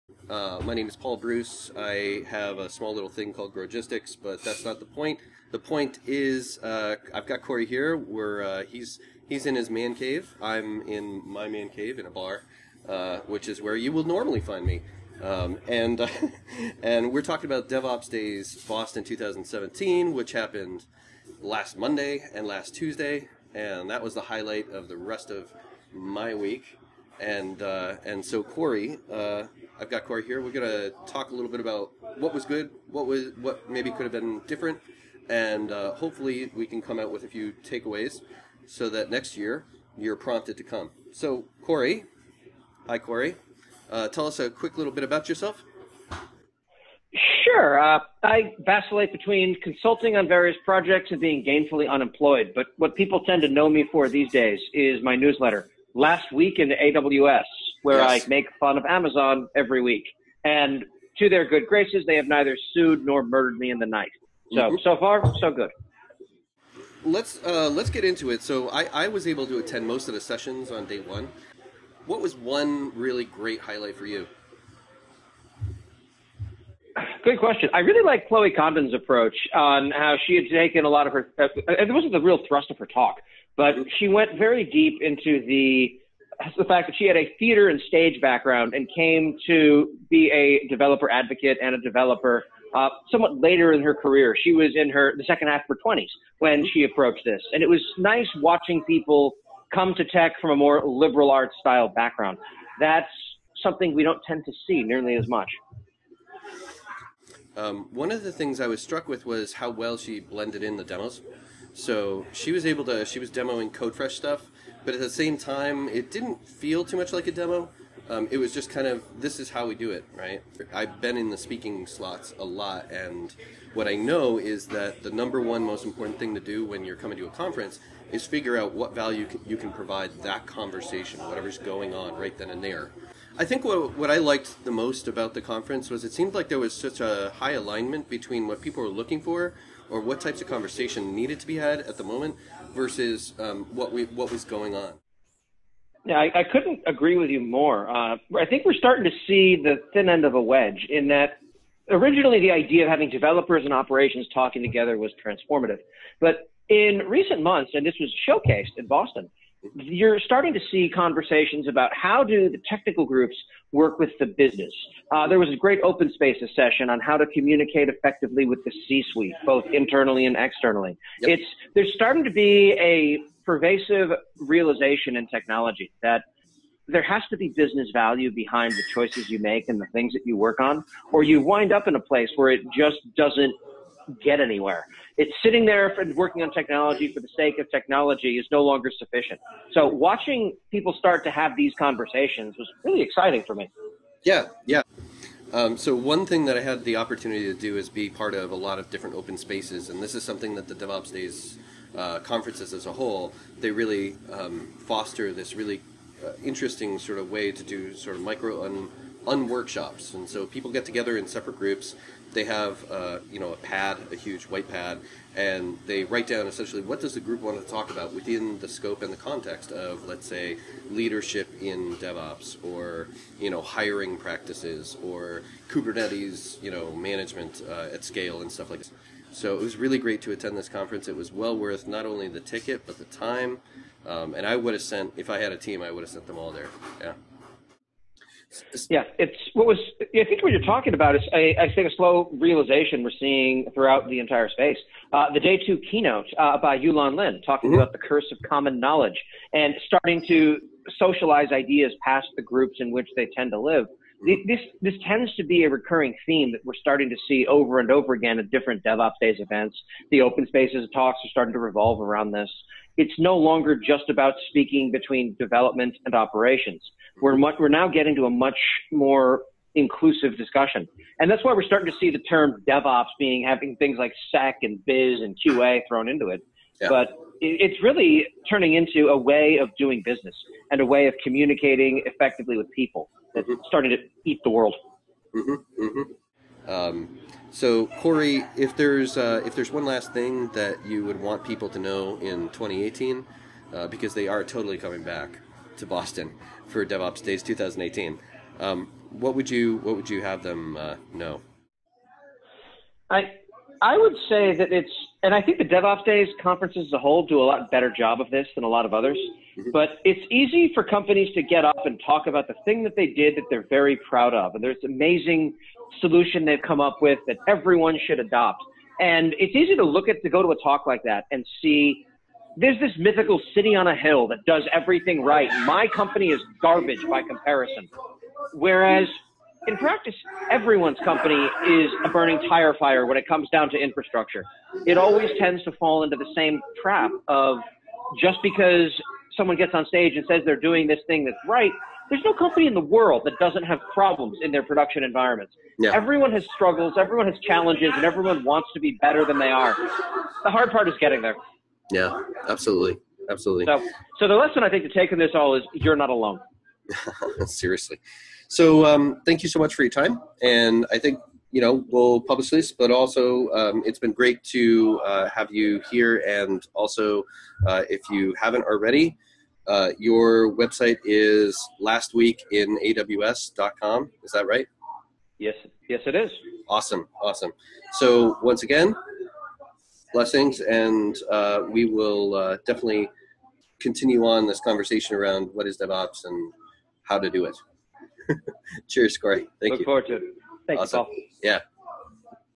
DevOps Days Boston 2017 - Corey Quinn
A quick chat with Corey Quinn and Paul Bruce about DevOps Days Boston 2017.